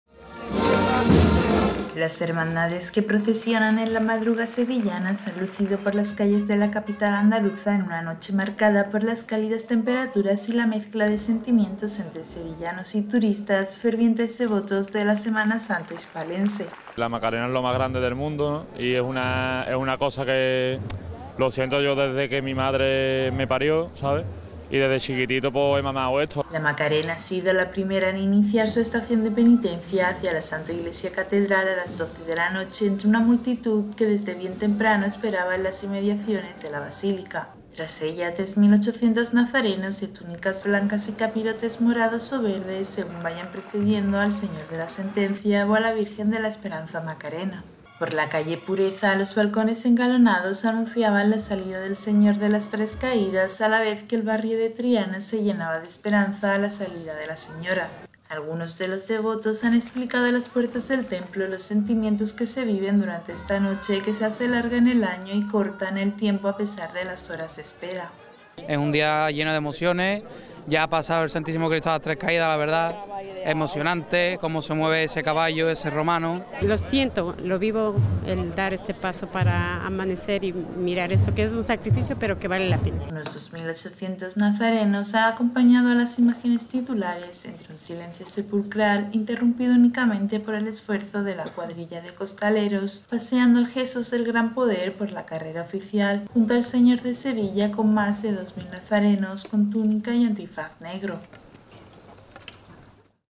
La ‘Madrugá’ sevillana se luce en una noche cálida marcada por los sentimientos
Las hermandades que procesionan en la 'Madrugá' sevillana se han lucido por las calles de la capital andaluza en una noche marcada por las cálidas temperaturas y la mezcla de sentimientos entre sevillanos y turistas, fervientes devotos de la Semana Santa hispalense.